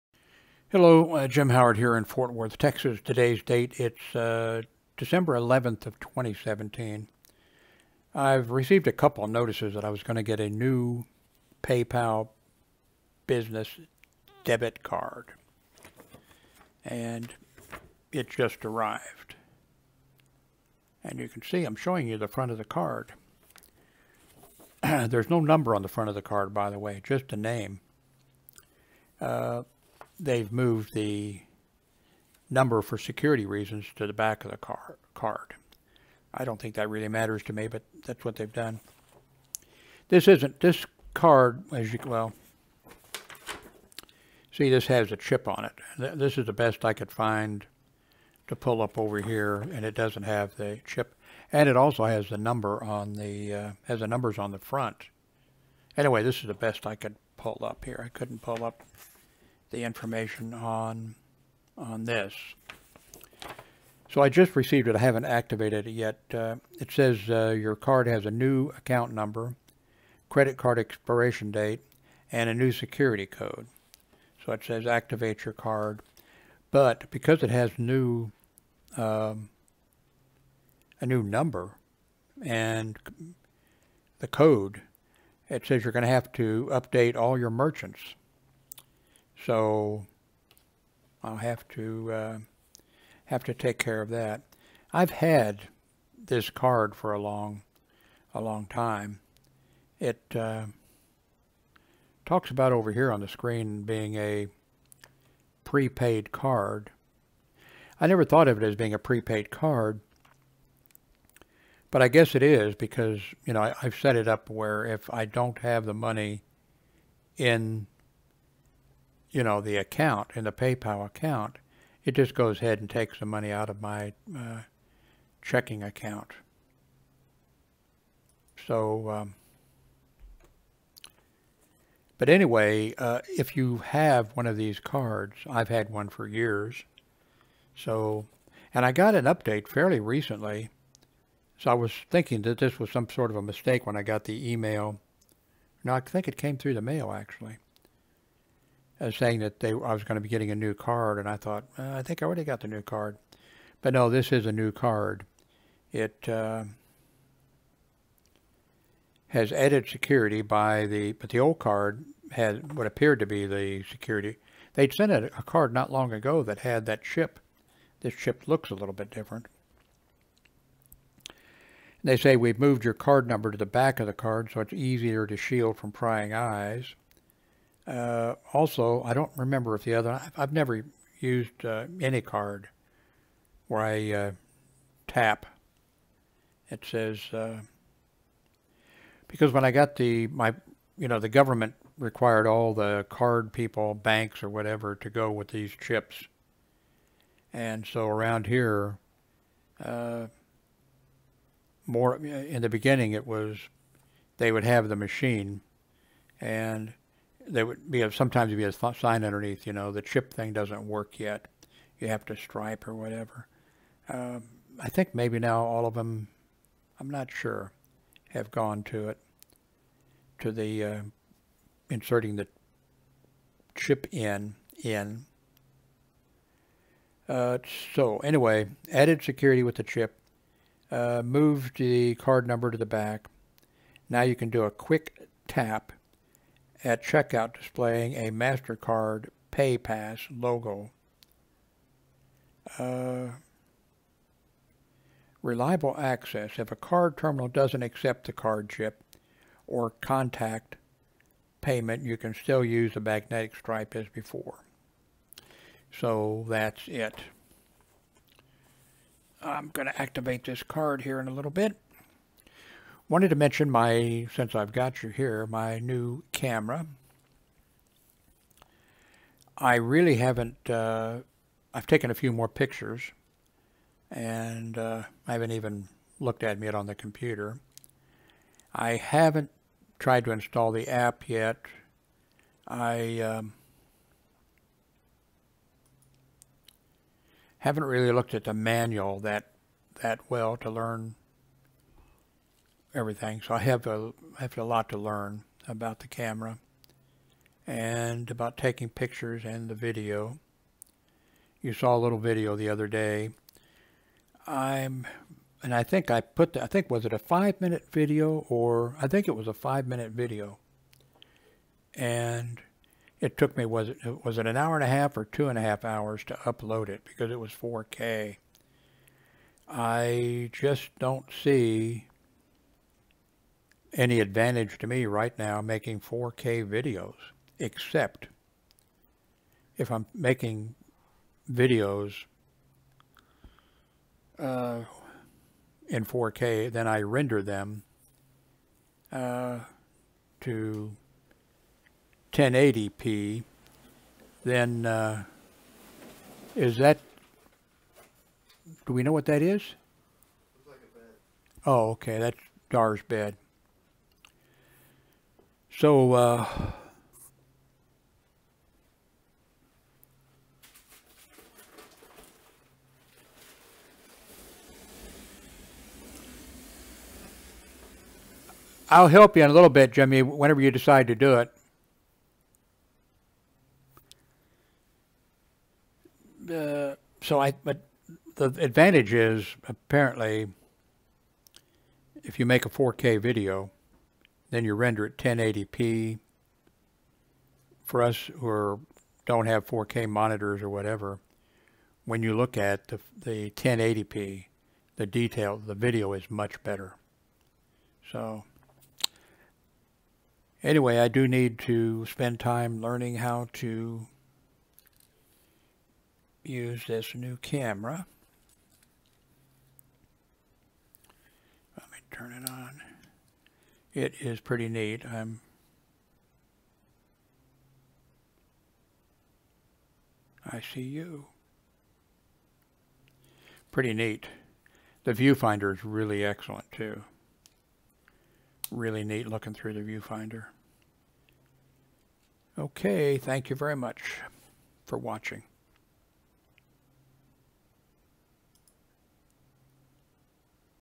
The replacement PayPal Business Debit Mastercard
PayPal has made a change to their PayPal Business Debit Mastercard.
I received my new PayPal card today.  It replaces the old card.